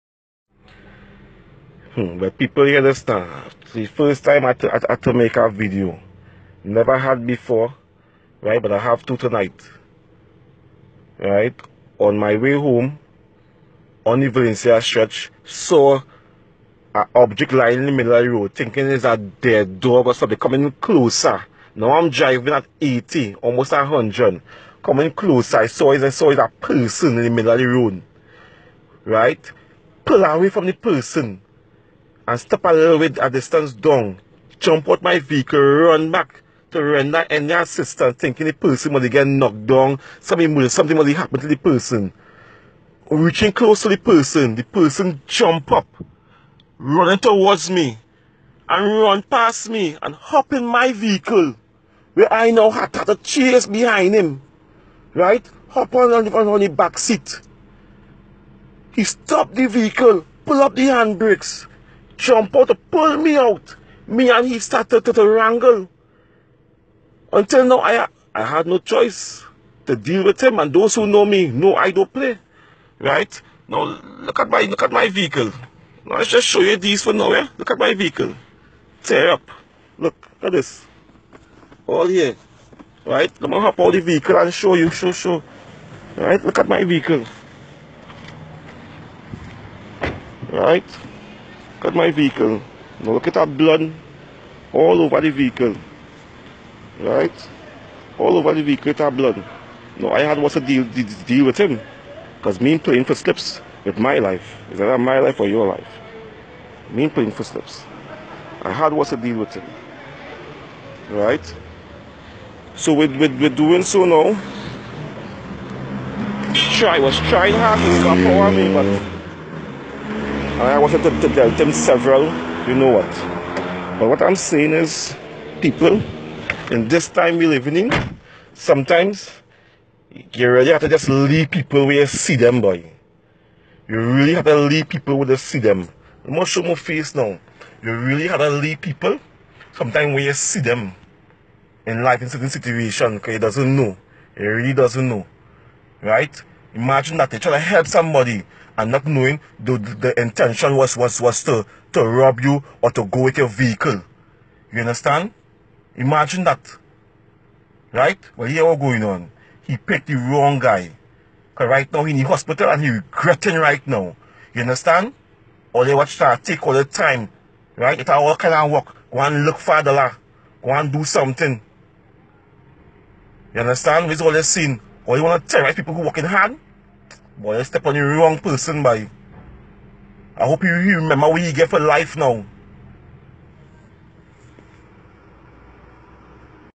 Valencia attempted car jack.
This guy describes how he dealt with an attempted robbery and offers advice to fellow road users.